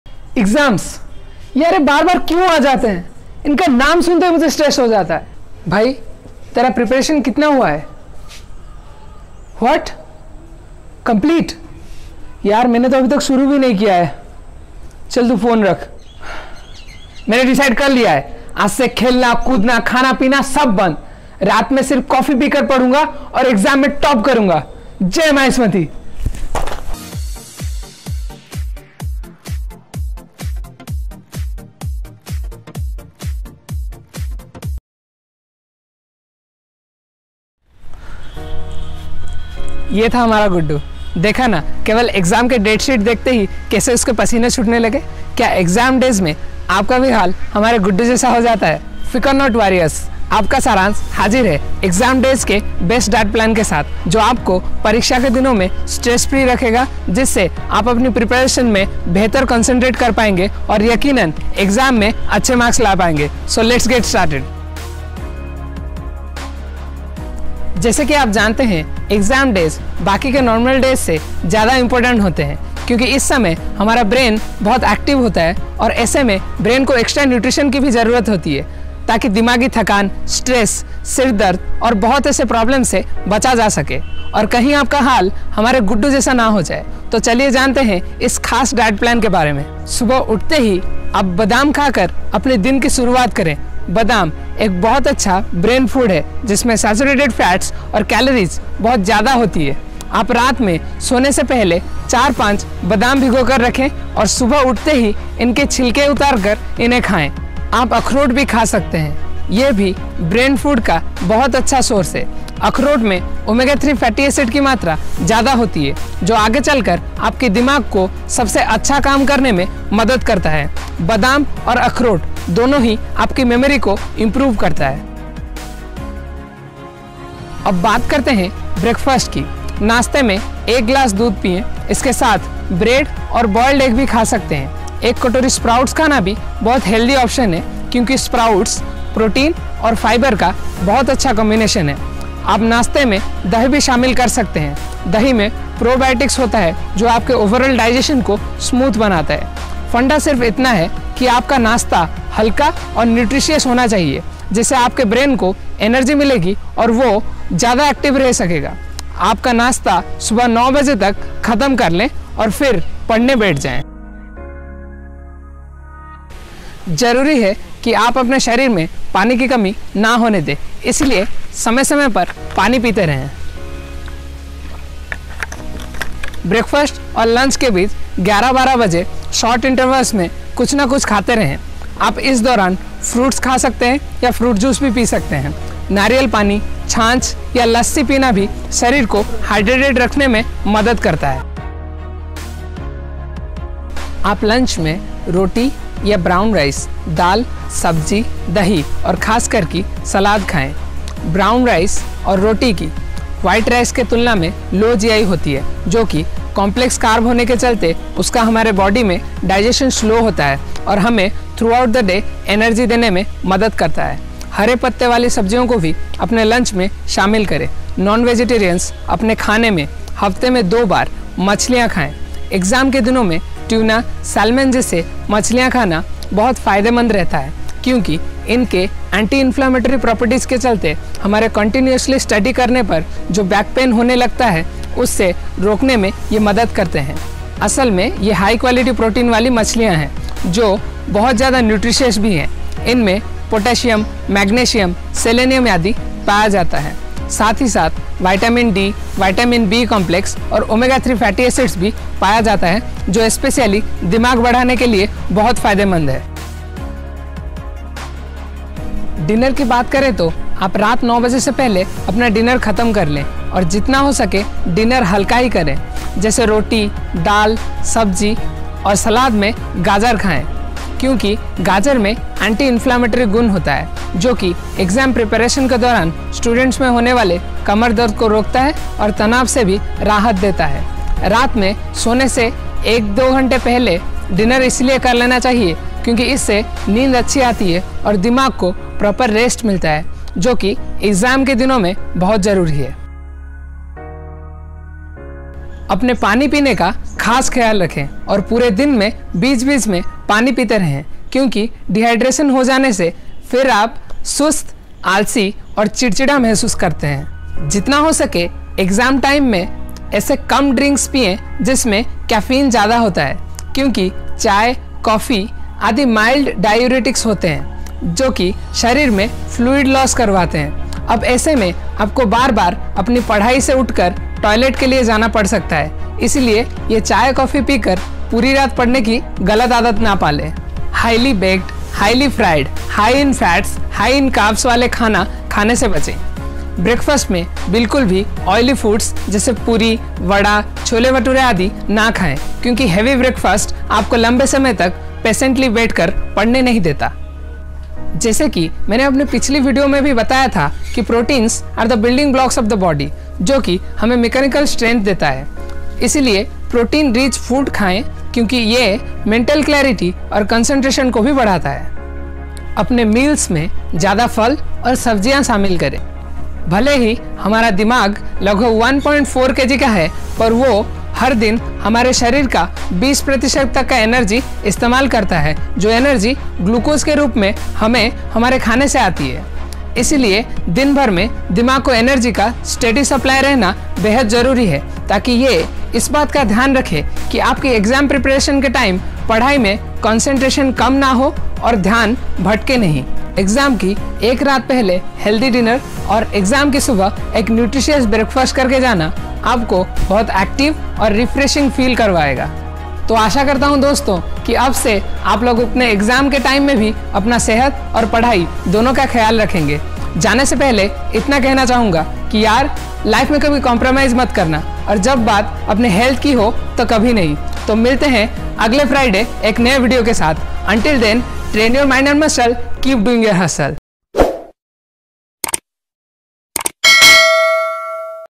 Best Diet Plan During Exams | Beyond Fitness Student's Exam Guide
🎁 Attend My ₹999 Weight-Loss Masterclass for FREE -

Welcome Back, Warriors!
We all know when it comes to exams, students often neglect their health and hence underperform in their exams even after so much preparation. And that is why Team Beyond Fitness is here with the BEST DIET PLAN FOR EXAM DAYS which will help you eat your way towards success.
We have talked about the right food, drinks that should be eaten during exam days for better memory, alertness, and faster information processing. We have also explained the respective nutritional benefits and health benefits of such foods on students during these days. Some important tips are also covered towards the end of the video that needs to be kept in mind for stronger academic performance.
Hope you follow this diet and give your best in your exams.
Best wishes
Team Beyond Fitness

CHAPTERS
0:00 Intro
1:29 Morning Diet After Waking Up
2:05 Breakfast
2:58 Mid Day Snacking
3:12 Lunch
4:17 Dinner
4:55 Important Tips

TEAM BEYOND FITNESS
VIDEOGRAPHY - Padmashree Samantaray
SCRIPTS - Shree Jaya Laxmi
CONCEPTS- Saransa Sourav
POST PRODUCTION- Girish Chandra Rath & Saransa Sourav
Regards
Beyond Fitness